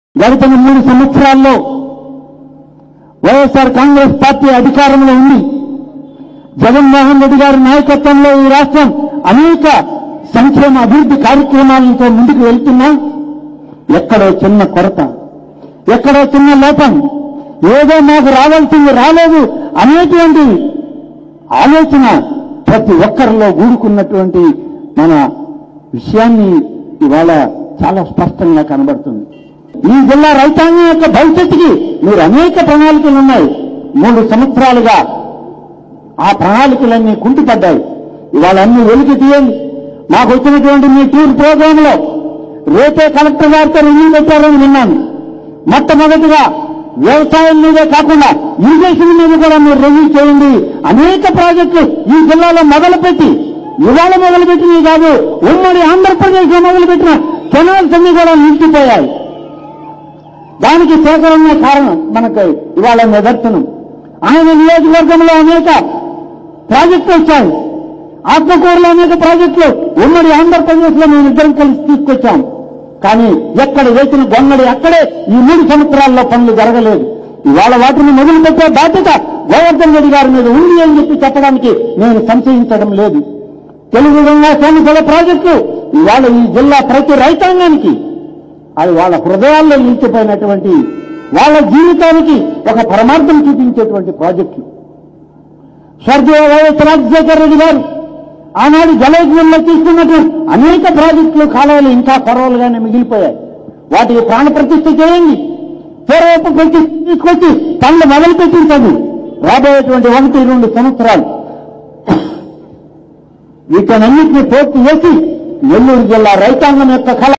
జగన్ పై ధ్వజమెత్తిన వైసీపీనేత ఆనం రామనారాయణ రెడ్డి | గడిచిన 3 years లో జగన్ చేసింది ఏమిలేదు
గడిచిన మూడు సంవత్సరాల జగన్ పాలనలో నీటిపారుదల ప్రాజెక్టుల పరిస్థితిపై ధ్వజమెత్తిన సొంత పార్టీ ఆనం రామనారాయణ రెడ్డి